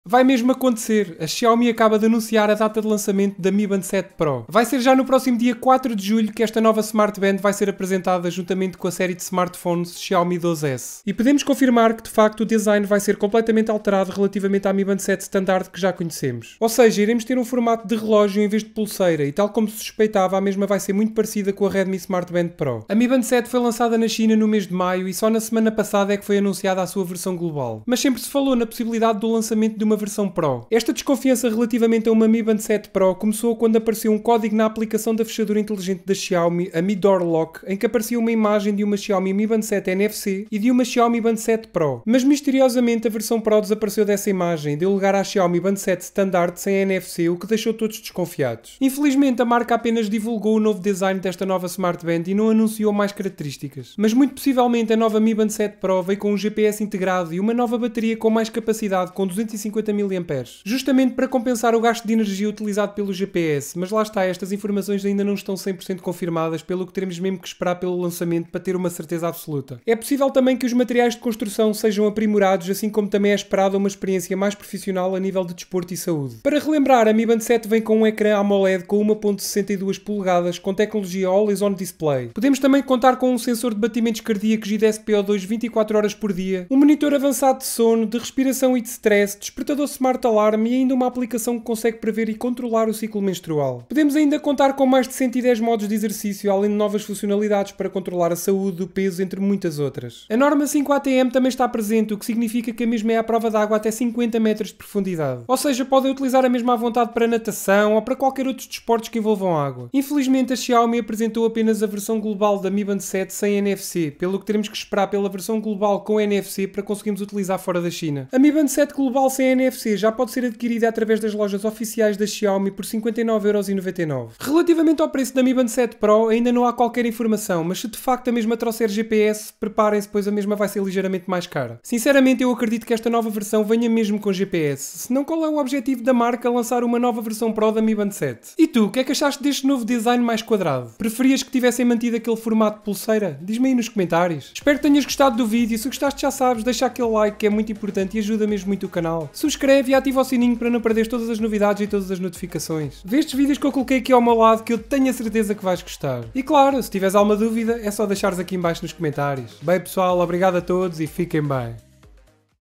CONFIRMADO! Xiaomi Mi Band 7 PRO tem Data de Lançamento OFICIAL
A Xiaomi acaba de anunciar a Data de Lançamento da Mi band 7 Pro. Vai ser já no próximo dia 4 de Julho que esta nova Smart Band vai ser apresentada juntamente com a série de smartphones Xiaomi 12s
Mi Band 7
Mi Band 6
Mi Band 6 NFC
Mi Band 5

Huawei Band 6

Amazfit Band 5
Redmi Smart Band Pro